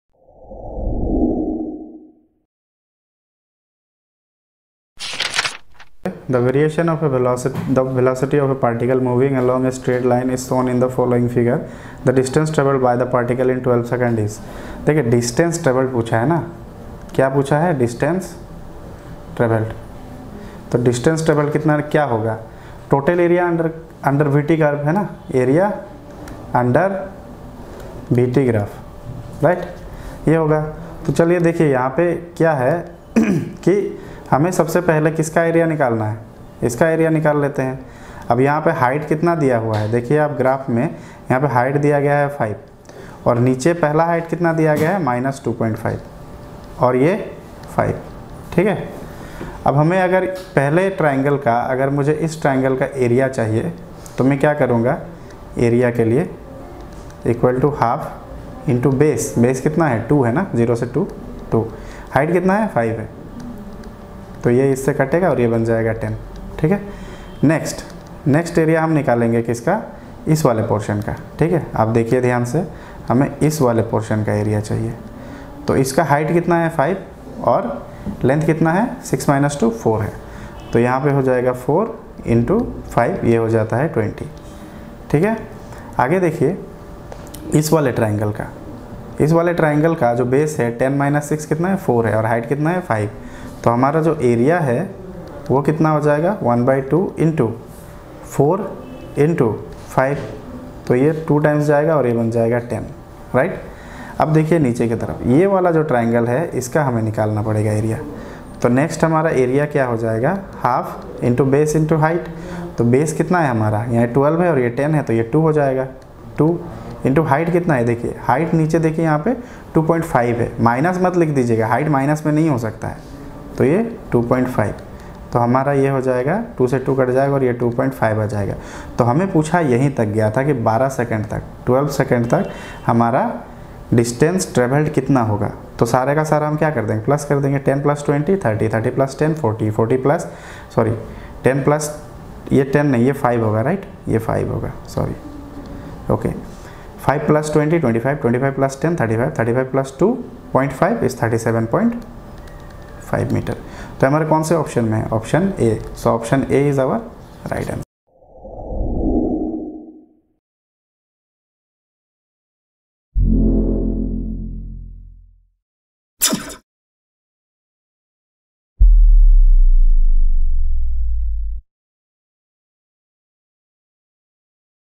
Q45. The variation of velocity of a particle moving along a straight line is shown in the following
Q45. The variation of velocity of a particle moving along a straight line is shown in the following figure. The distance travelled by the particle in 12s is :  (1) 37.5 m (2) 32.5m (3) 35.0m (4) None of these.